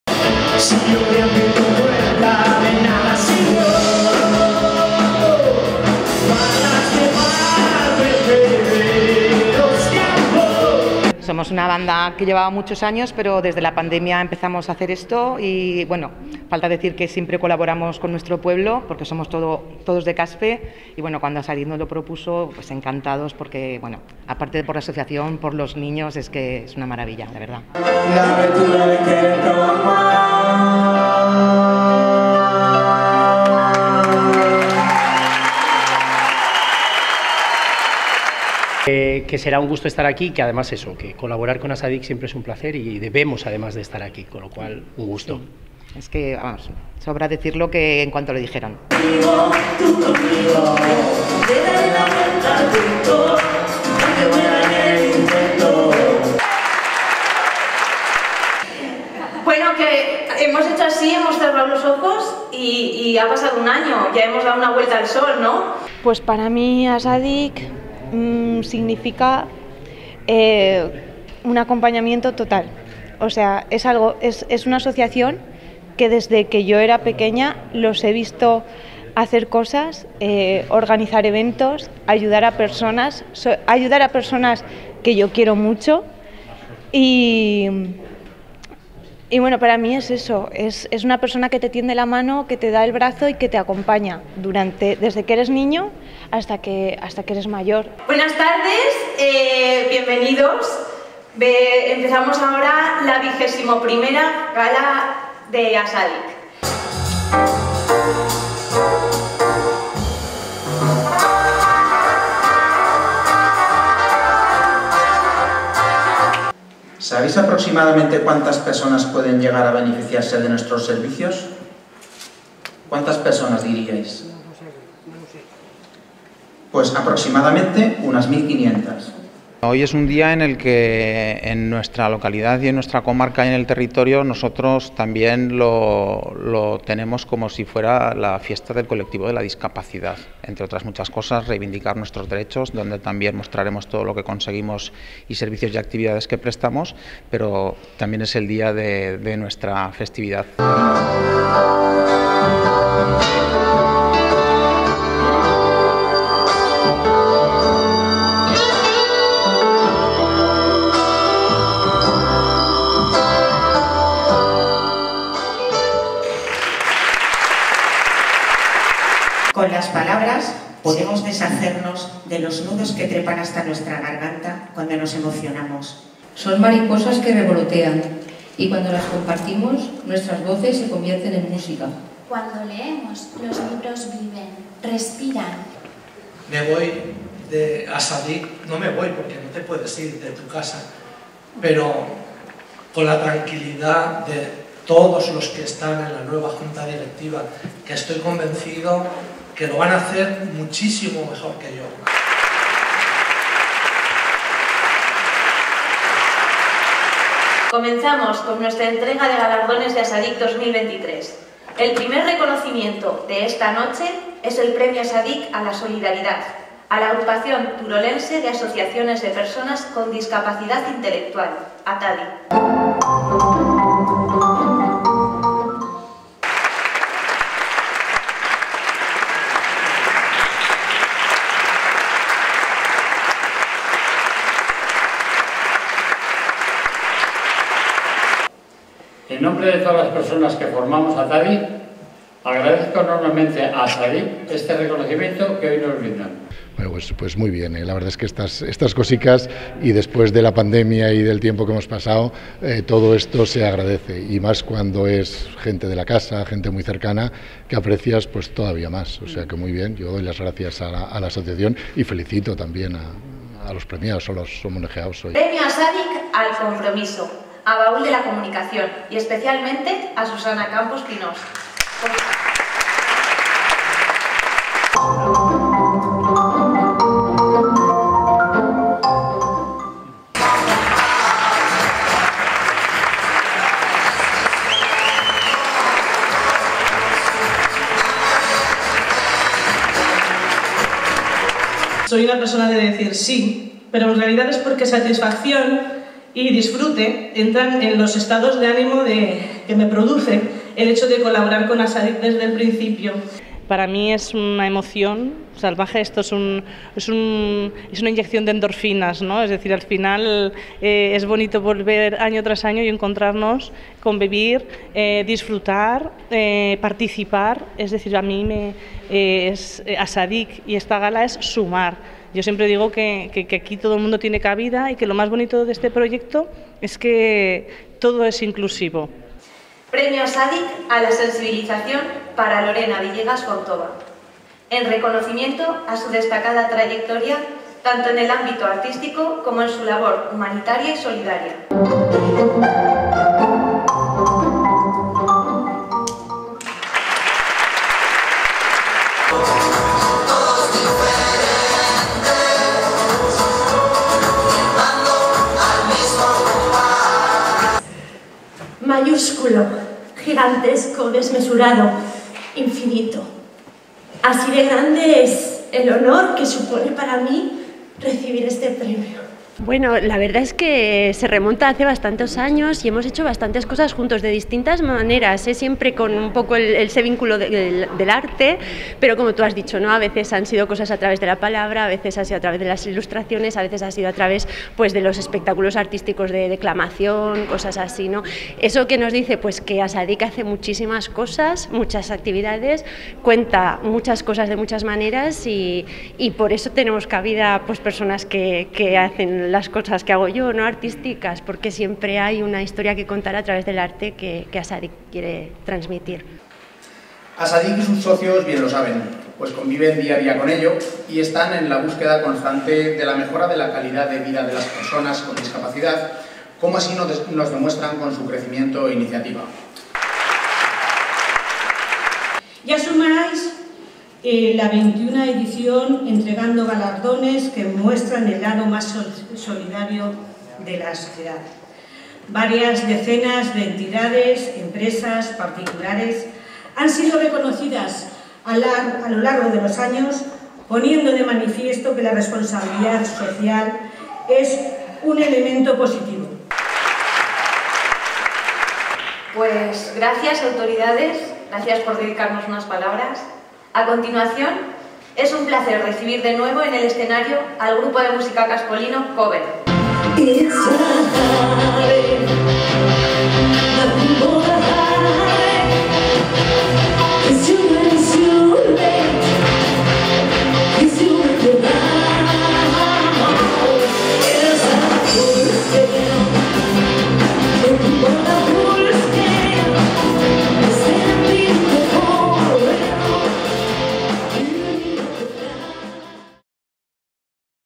XXI Edición Gala PREMIOS ASADICC | Caspe 2023
Caspe celebró durante la noche del 9 de junio de 2023 la XXI edición de los Premios Asadicc en el Teatro-cine Goya. Una gala llena de emociones que presentó Lucía Bel y que abrió y cerró la banda local Cover. Los galardonados fueron, ATADI premio a la Solidaridad, El Baúl de la Comunicación, premio al Compromiso y Lorena Villegas Fontoba, premio a la Sensibilización.